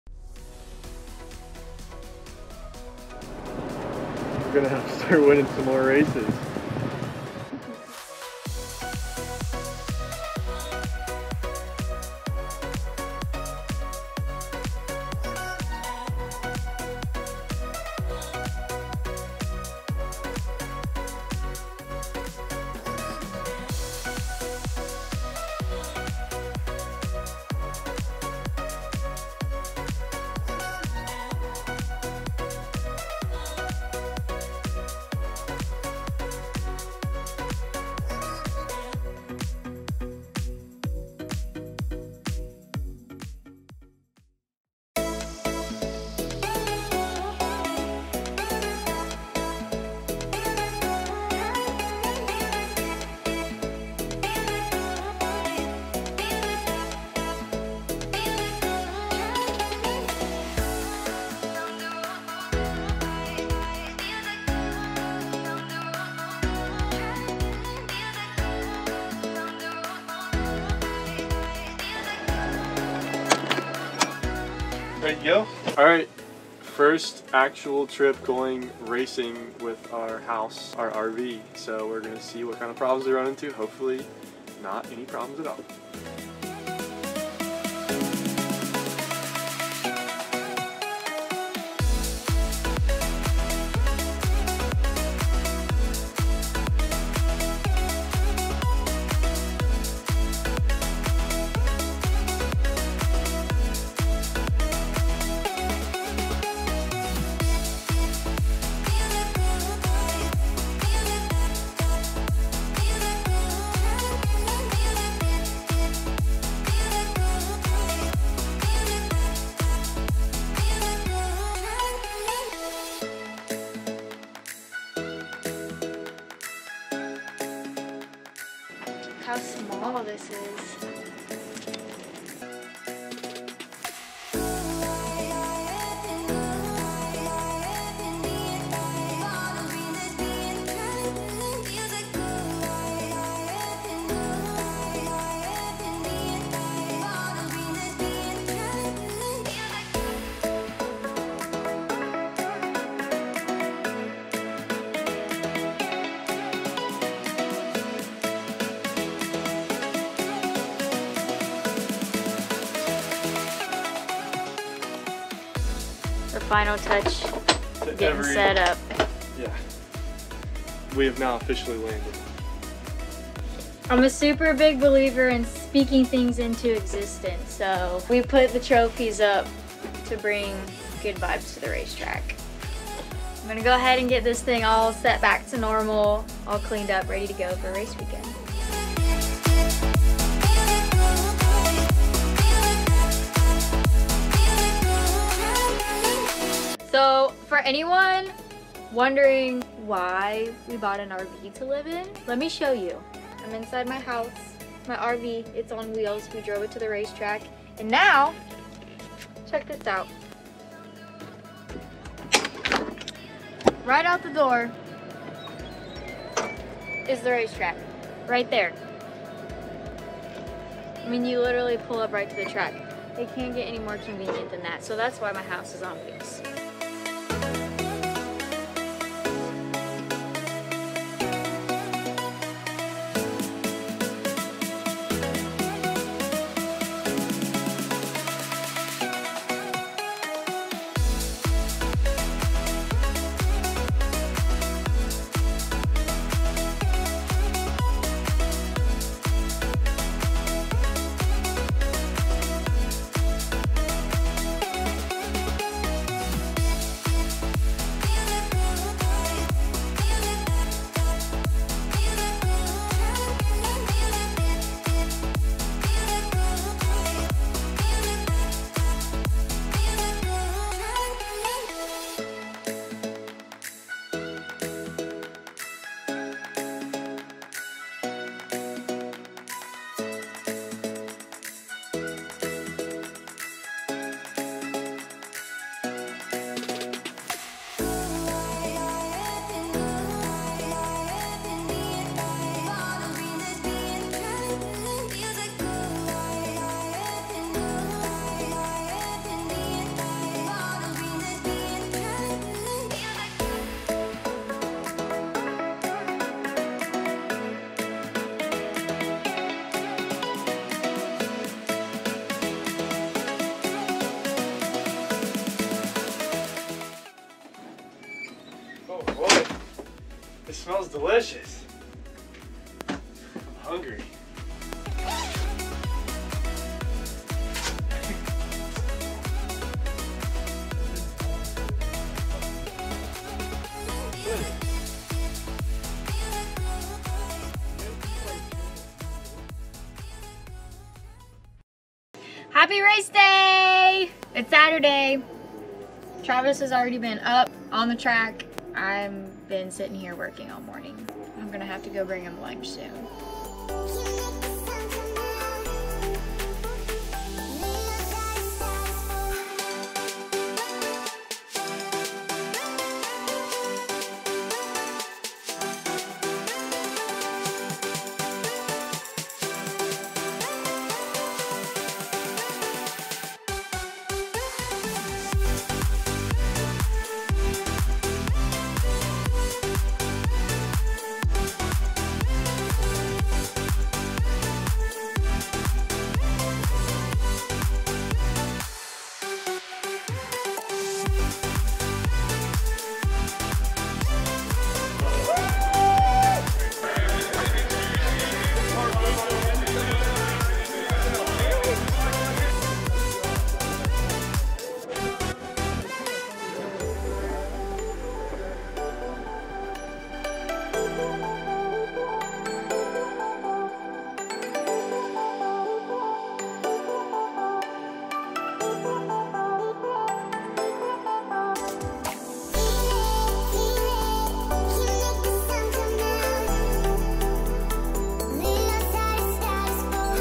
First Trip in the RV // Weekend at the Racetrack
This was our first trip as full time RVers! We drove to Southern National Motorsports Park in our new Class A motorhome. I competed in the CARS Tour season opener and brought home a top 5 finish!